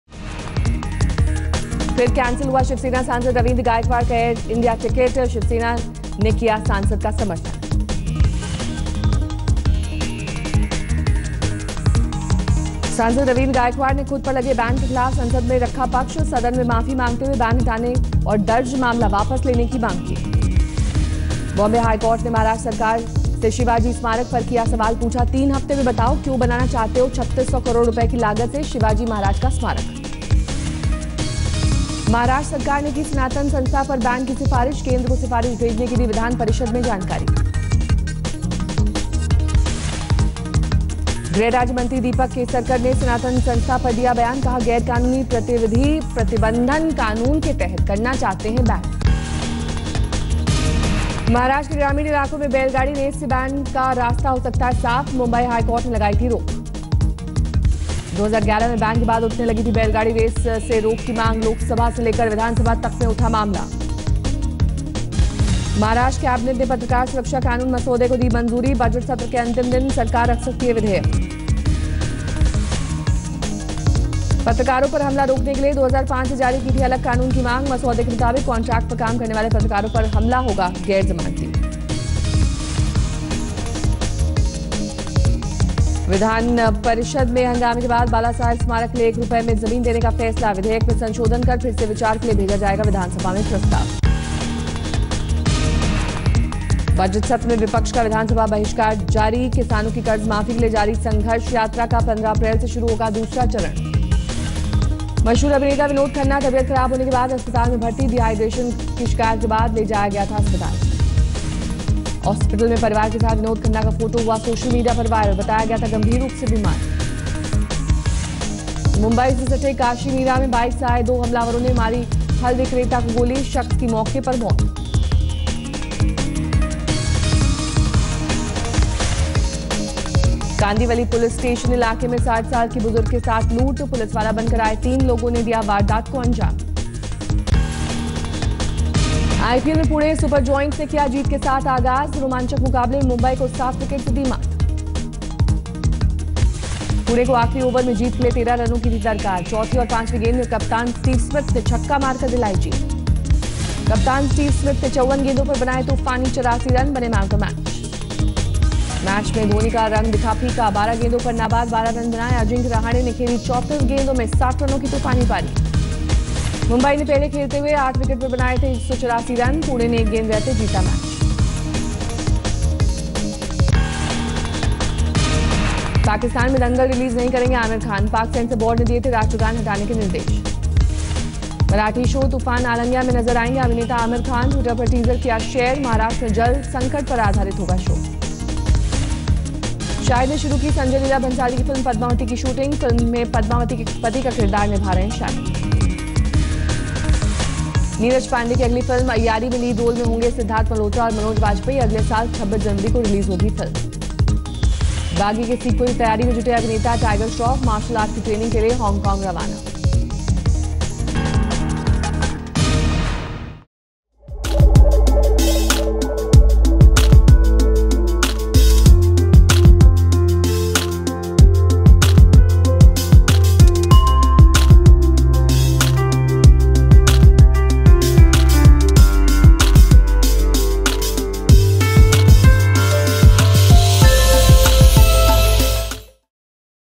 Mumbai 25 Khabare: High Court Seeks Maharashtra Govt Stand On Plea Against Shivaji Memorial
Watch out for the top headlines of the day, where you will get updates on:
Yogi Adityanath push for construction of an international airport at Jewar.
Rajya Sabha passes all four GST bills without amendments
Will boycott NDA meet if Gaikwad issue not resolved by April 10, says  Shiv Sena
RBI chief hits out at Yogi Govt's farm loan waiver
Steve Smith  shines as Rising Pune Supergiants beats Mumbai Indians
Imran Tahir takes three wickets
Farm loan waivers a bad idea: RBI governor Urjit Patel
Shiv Sena MP Ravindra Gaikwad finally regrets assaulting Air India officer
Schools, Colleges Close, Highway Blocked After Rare April Snowfall In Jammu And Kashmir
Yogi Adityanath Scraps Samajwadi schemes in Uttar Pradesh
Mayawati accuse BJP of trying to convert India into a Hindu Rashtra
UP CM Yogi Adityanath Wants All Doctors In UP To Work In Villages For Two Years and more..